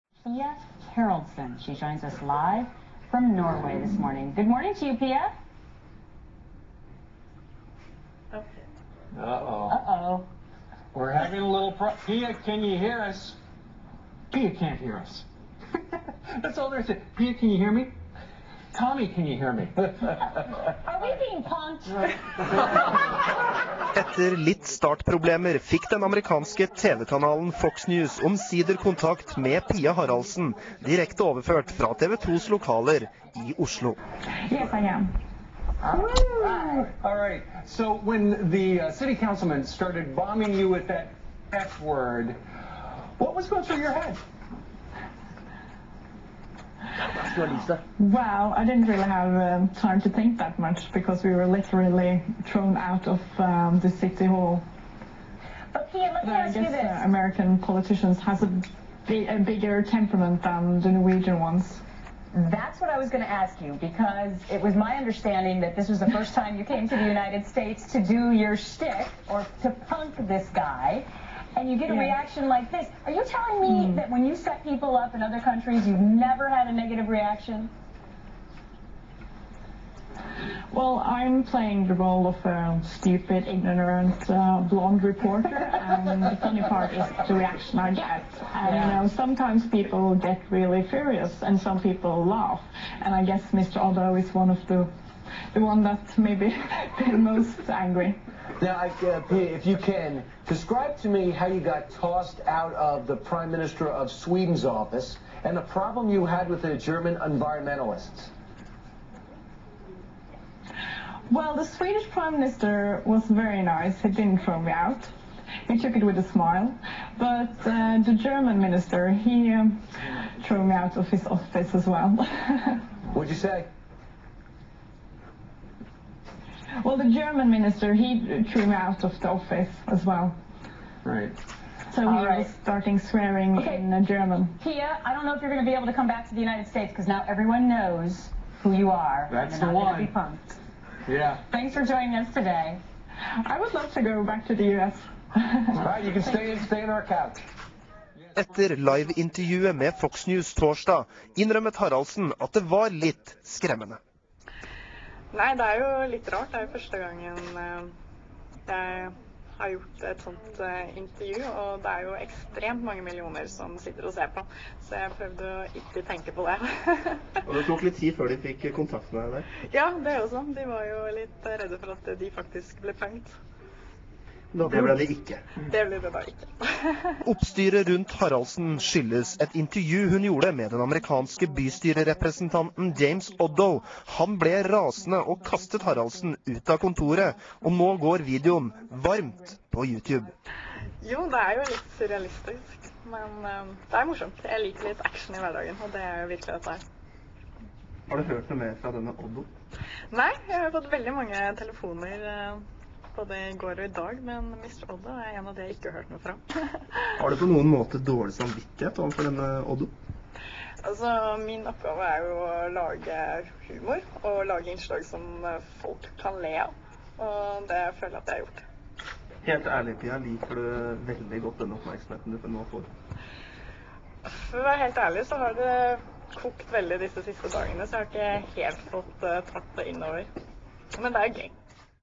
Pia Haraldsen på FoxNews
Rikets Røst-reporteren ble intervjuet i forbindelse med et intervju hun gjorde med den amerikanske bystyrerepresentanten James Oddo.